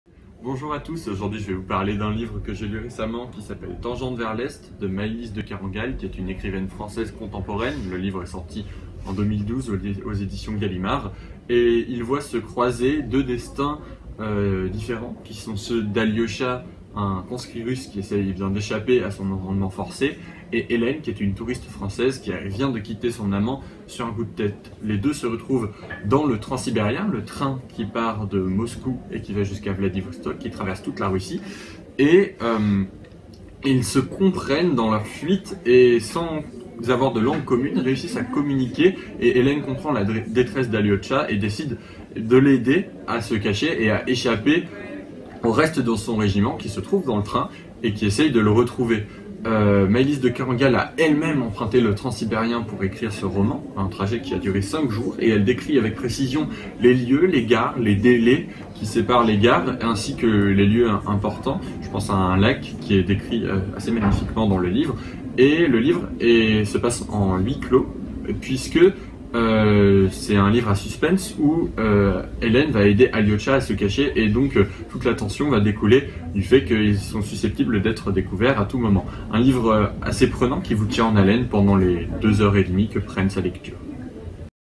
Tangente Vers L'est - Maylis de Kerangal (Verticales) - Printemps du livre
Booktube réalisé par des élèves d'hypokhagne du Lycée Champollion dans le cadre du Printemps du livre.